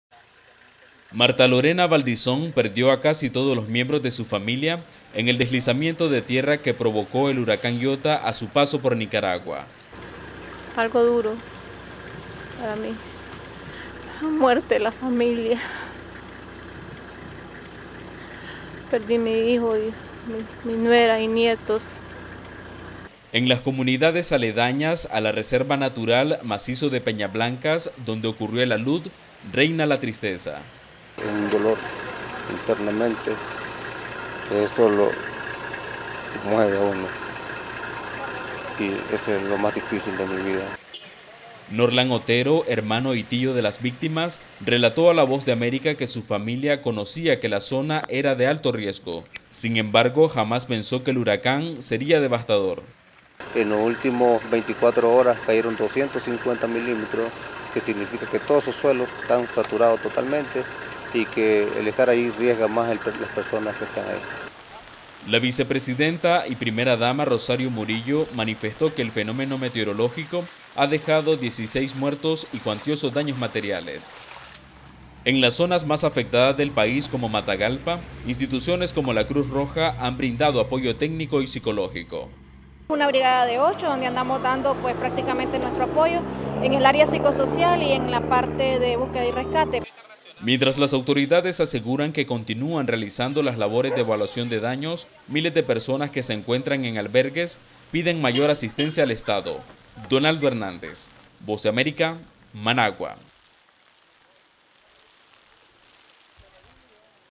Nicaragua: Asciende el número de víctimas del huracán Iota
En Nicaragua, asciende a 16 el número de víctimas fatales provocadas por el paso del huracán Iota. Mientras, las autoridades continúan con las labores de evaluación de daños, miles de personas en albergues piden mayor asistencia. Donaldo Hernández, con los detalles.

¡Visítenos!